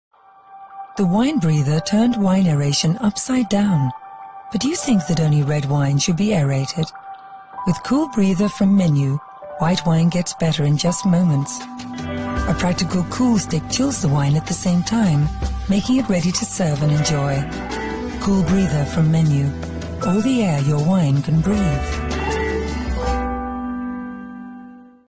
Karafka do białego wina firmy Menu
Doskonałym reprezentantem Autentycznego Skandynawskiego Designu są karafki cool breather do białego wina -- piękne, pomysłowe i praktyczne! Polecamy!
Dostępne do kupienia w sklepie Kondrat Wina Wybrane przy ulicy Wierzbowej 3 w Warszawie